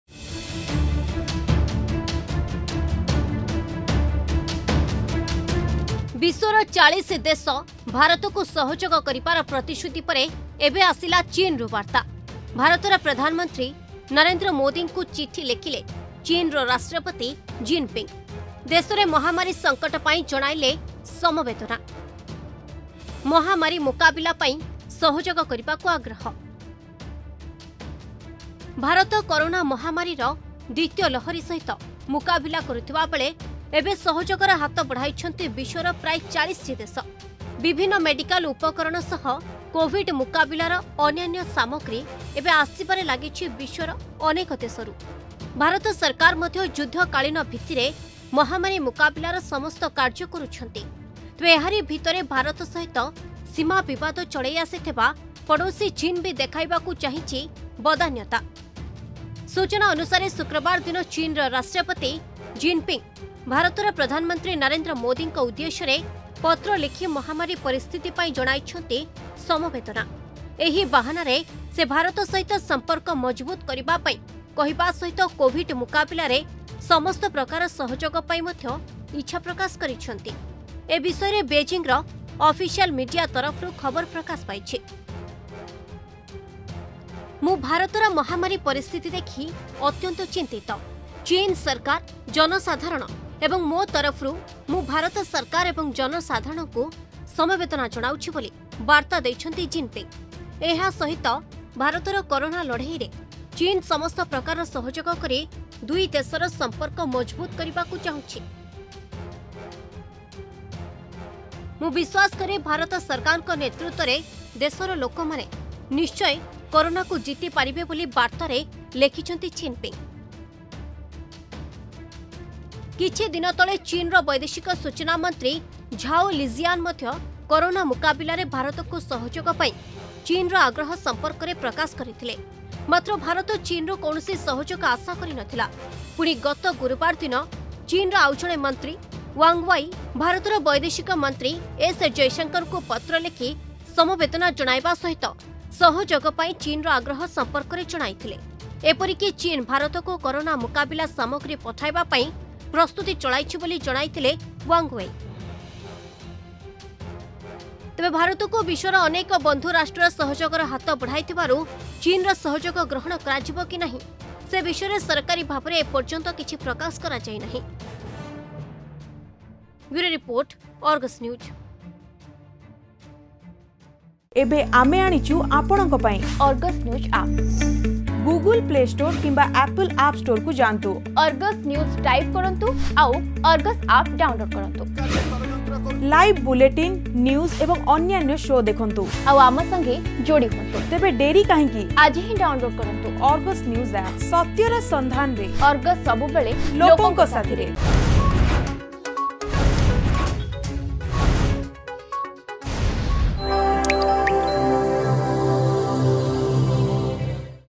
ମୋଦିଙ୍କୁ ଚିଠି ଲେଖିଲେ ଜିନ୍‌ପିଙ୍ଗ୍‌
ଭାରତର ପ୍ରଧାନମନ୍ତ୍ରୀ ନରେନ୍ଦ୍ର ମୋଦିଙ୍କୁ ଚିଠି ଲେଖିଲେ ଚୀନର ରାଷ୍ଟ୍ରପତି ଜିନ୍‌ପିଙ୍ଗ୍‌। ଦେଶରେ ମହାମାରୀ ସଙ୍କଟ ପାଇଁ ଜଣାଇଲେ ସମବେଦନା.